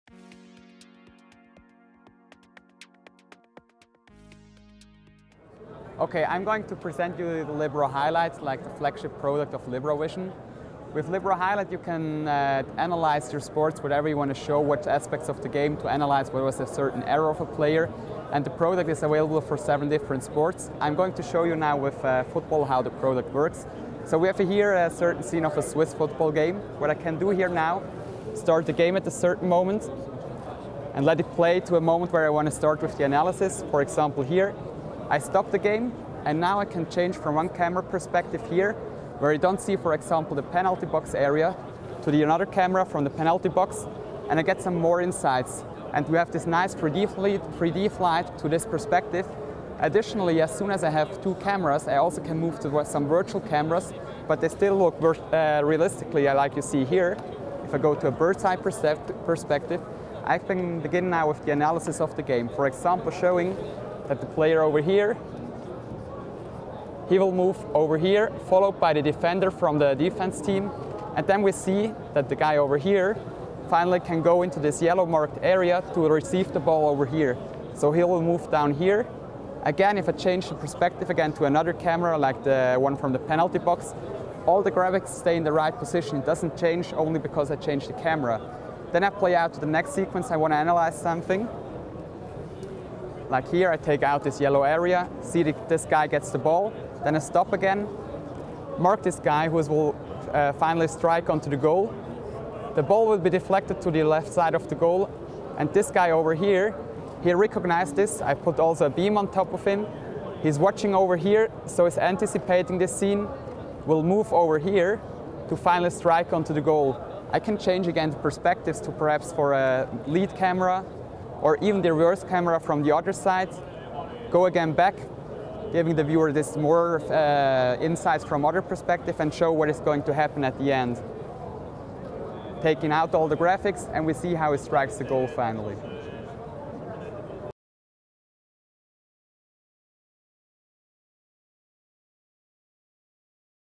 LiberoVision at NAB 2011 - advanced sports analysis
Vizrt and LiberoVision demonstrate just how easy it is to enhance your half time analysis with no additions to your existing broadcast infrastructure.